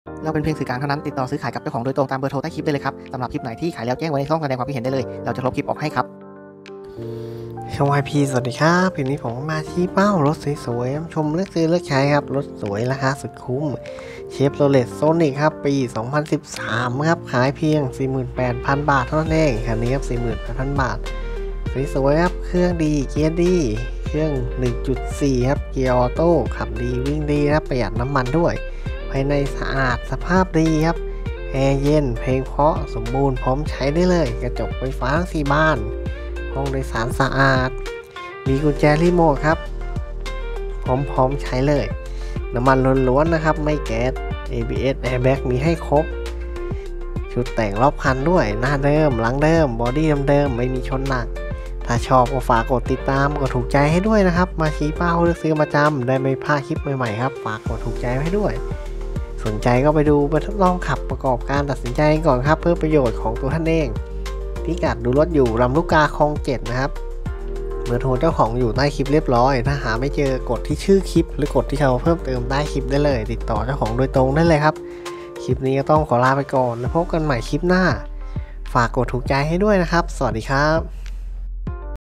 👑ขายด่วน 48,000 บาท รถสวยกิ๊บ🎀🎉เดิมๆโรงงาน👍
🌟🌟🌟🌟ขายแล้ว  · 🌟🌟🌟🌟
2013 Chevrolet sonic
฿48,000

คำอธิบายของผู้ขาย
Chevrolet Sonic LT ปี2013
-เครื่องเดิมecotec1.4 เกียร์ออโต้
-แอร์เย็น เพลงเพราะ
-ภายในสะอาดเบาะหนัง
-กุญแจรีโมท
-กระจกไฟฟ้า
-สีสวยเงารอบคัน80%
-ไช้น้ำมันล้วน
-airbag/abs
-ชุดแต่งรอบคัน
-ไมล์น้อย 11x,xxxกม.
รถพร้อมใช้งาน ขับทางไกลได้สบาย
ภาษีไม่ขาด เอกสารครบพร้อมโอน

.ลองดู สินค้าอื่น